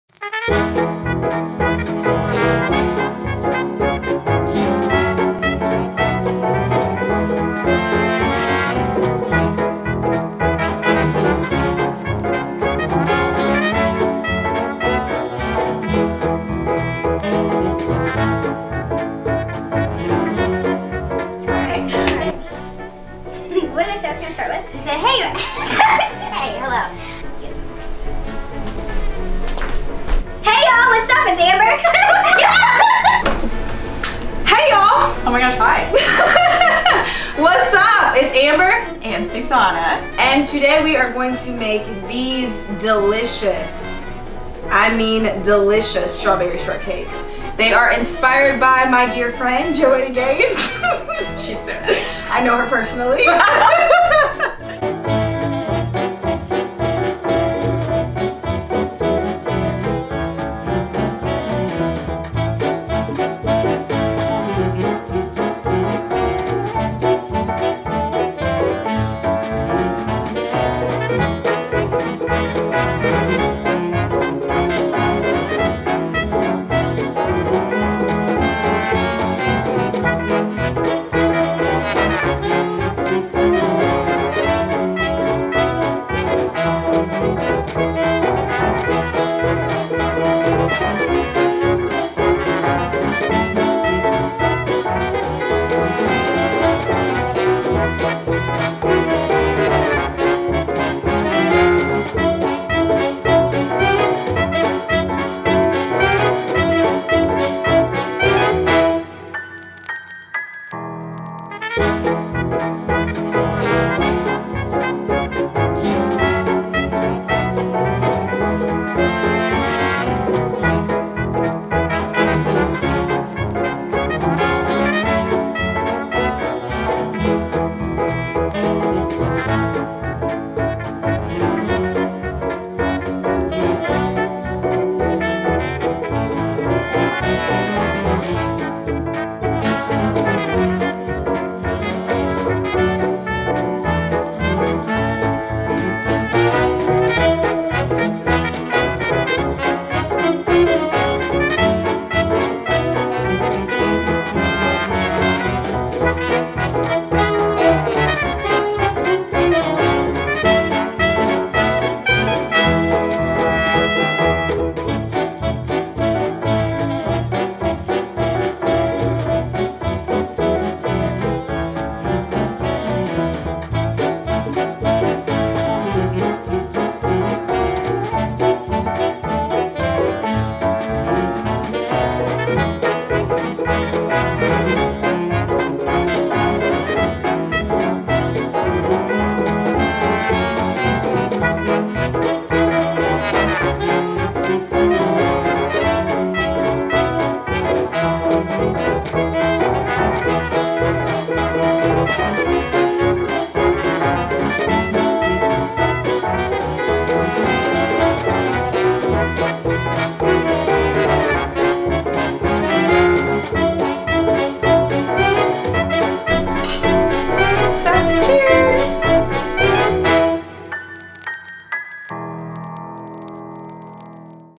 Strawberry Shortcake
Me and my friend, Susanna went strawberry picking. So naturally, we decided to make strawberry shortcakes. In this video, I will cook the strawberry shortcakes from Joanna Gaines cookbook, "Magnolia Table". See the ingredients below:

Biscuits:
4 cups of self-rising flour
2 TBS of baking powder
1 TSP of baking soda
3 sticks of salted butter
2 large beaten eggs
1.5 cups of buttermilk

Whipped Cream:
1 pint of heavy cream
1/4 cup sugar
1TSP pure vanilla extract

Strawberries:
2 lbs of strawberries
1/2 cup of sugar